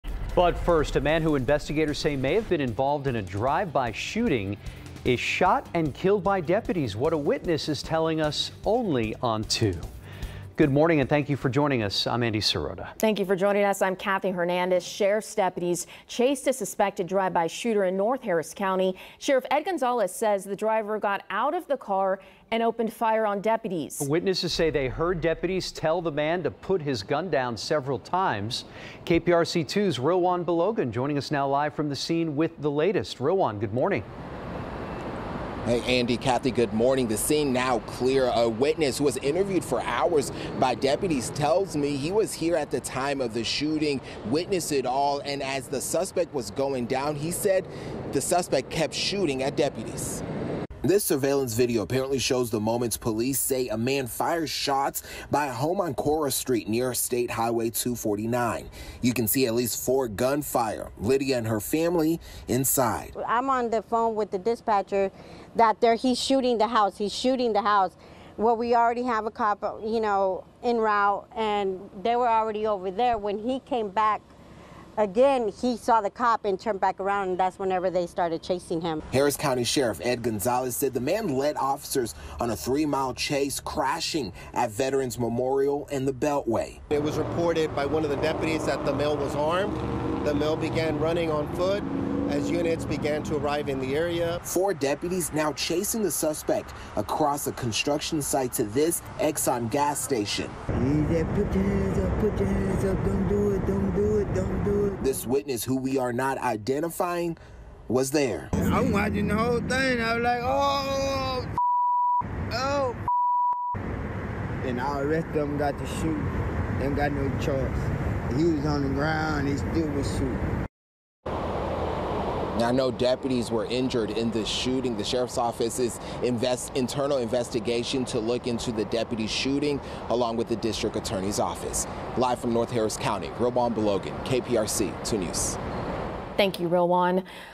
Possible drive-by shooting suspect killed after firing at deputies in north Harris County, HCSO ...
An investigation is underway after Harris County sheriff’s deputies said a man possibly involved in a drive-by shooting was fatally shot after firing at deputies in north Harris County Monday.